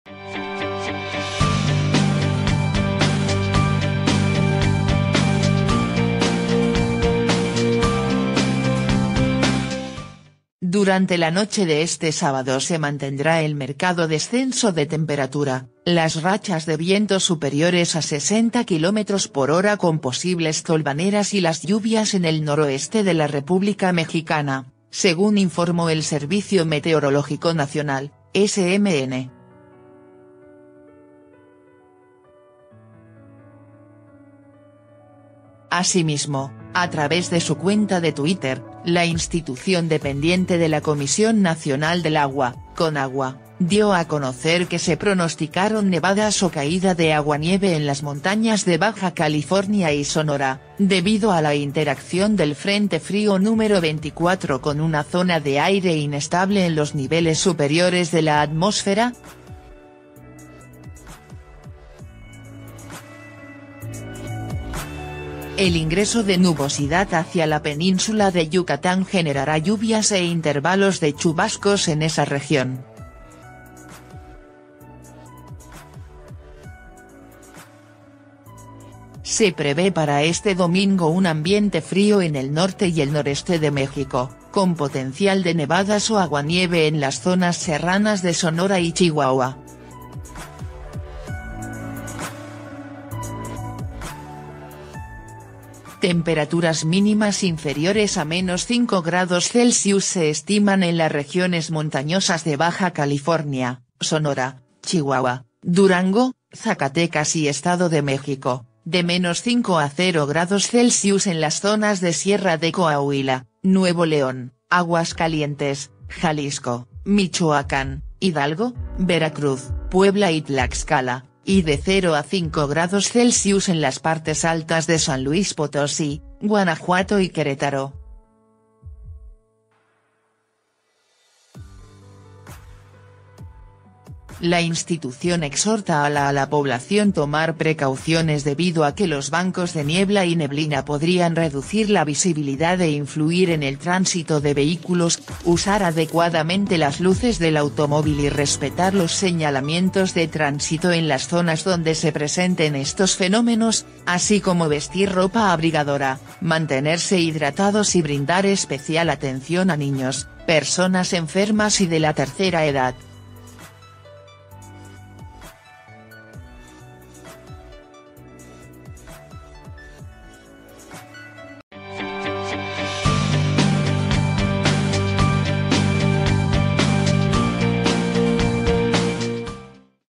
Persisten bajas temperaturas, vientos y lluvias
Durante la noche de este sábado se mantendrá el marcado descenso de temperatura, las rachas de viento superiores a 60 kilómetros por hora con posibles tolvaneras y las lluvias en el noroeste de la República Mexicana, según informó el Servicio Meteorológico Nacional (SMN).

Fuente: goo.gl/6K6t71